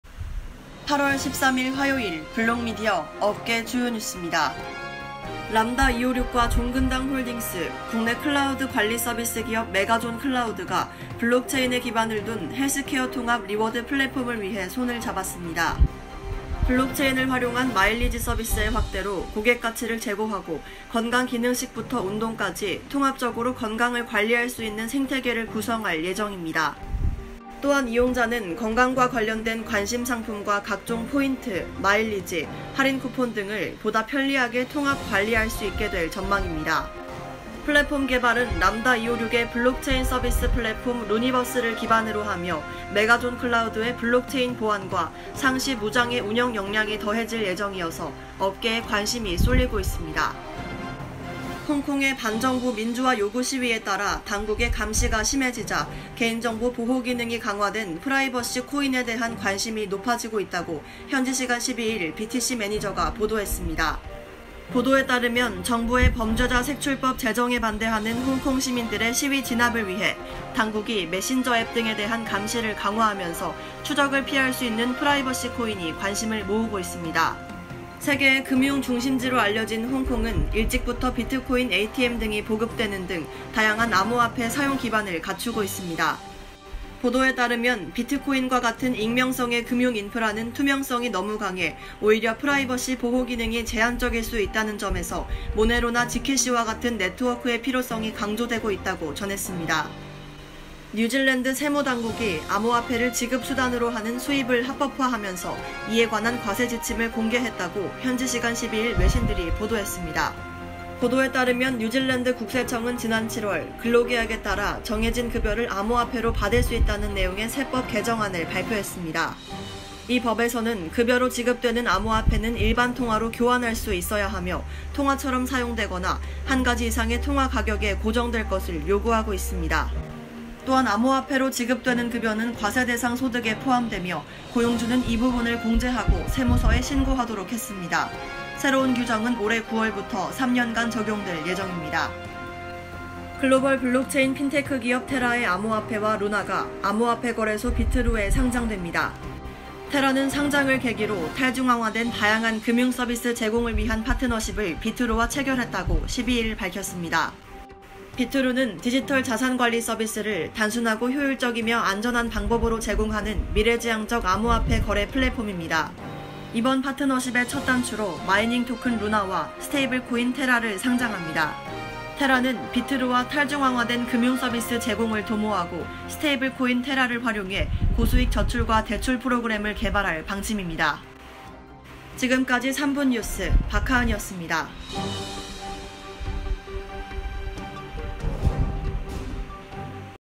3분 뉴스ㅣ8월 13일 화요일ㅣ종근당홀딩스 • 람다256 • 메가존클라우드, ‘블록체인 기반 헬스케어플랫폼’ 공동 추진
1. ‘람다(Lambda)256’과 종근당홀딩스, 국내 클라우드 관리 서비스 기업(MSP) 메가존클라우드가 블록체인에 기반을 둔 헬스케어 통합 리워드 플랫폼을 위해 손을 잡았습니다.

2. 홍콩의 반정부 민주화 요구 시위에 따라 당국의 감시가 심해지자 개인정보 보호 기능이 강화된 프라이버시 코인에 대한 관심이 높아지고 있다고 현지시간 12일 BTC매니저가 보도했습니다.

3. 뉴질랜드 세무당국이 암호화폐를 지급 수단으로 하는 수입을 합법화하면서 이에 관한 과세 지침을 공개했다고 현지시간 12일 외신들이 보도했습니다.

4. 글로벌 블록체인 핀테크 기업 테라의 암호화폐와 루나가 암호화폐 거래소 비트루(Bitrue)에 상장됩니다. 테라는 상장을 계기로 탈중앙화된 다양한 금융서비스 제공을 위한 파트너십을 비트루와 체결했다고 12일 밝혔습니다.